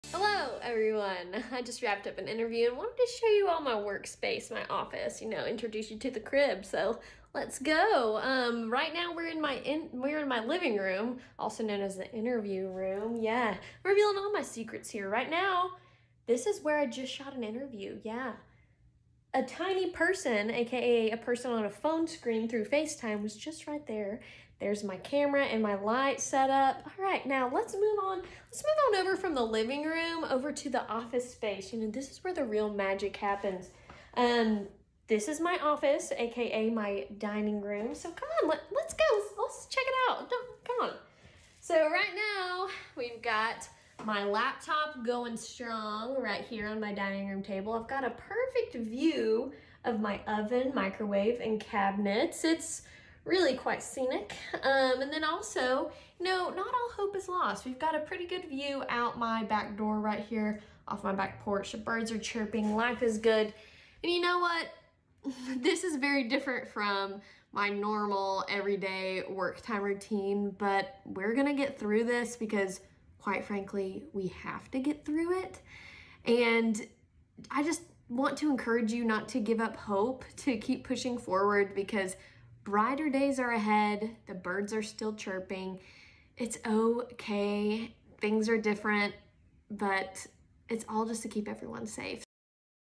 Working from Home: 10News reporter Katie Inman shows off her 'office'
Many 10News reporters, anchors and producers are working from home! Katie shows off her "office" -- which is also her kitchen table.